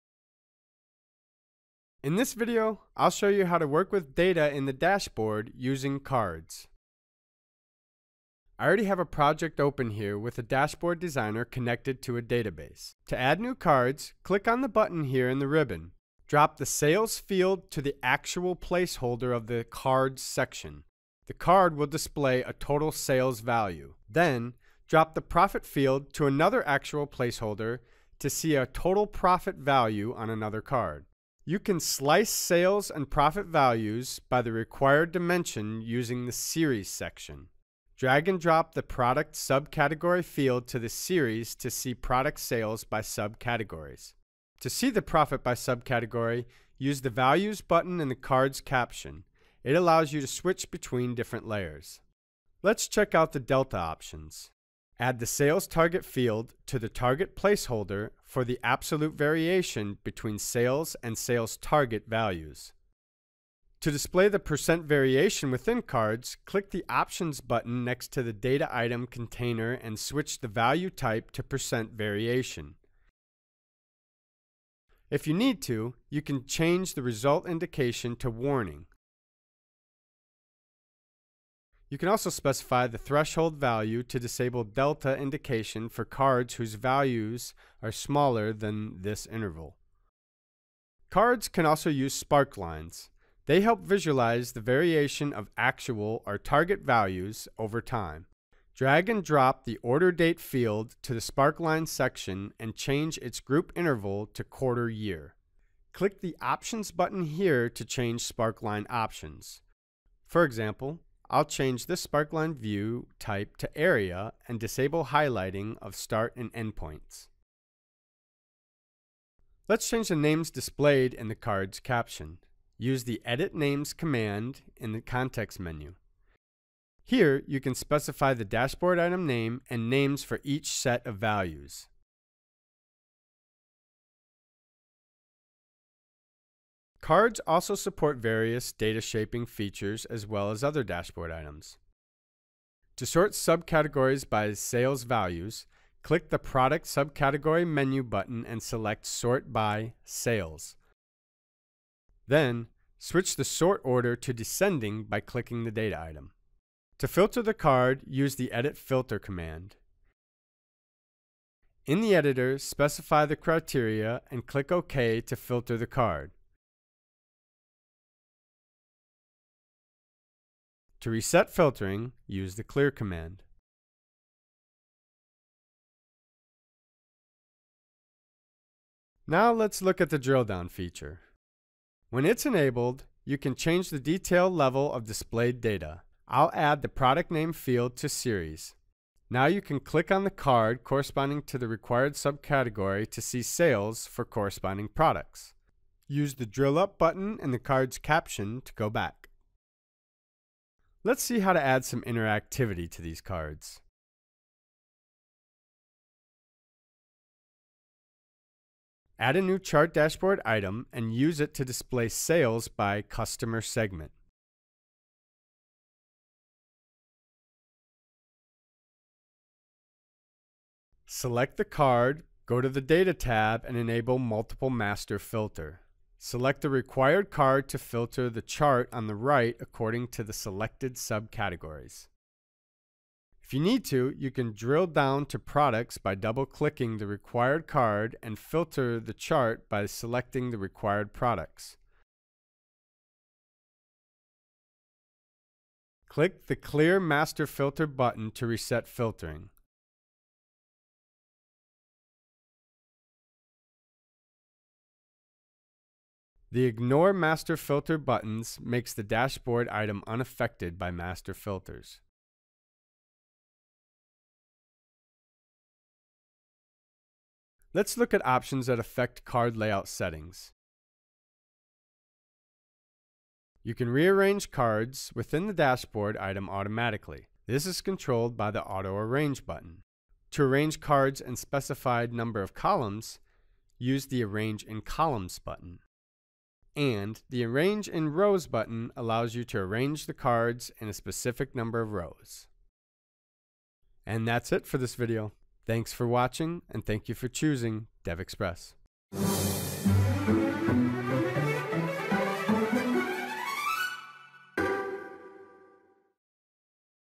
DevExpress Dashboards: Using Cards
This video show you how to work with cards inside of a dashboard.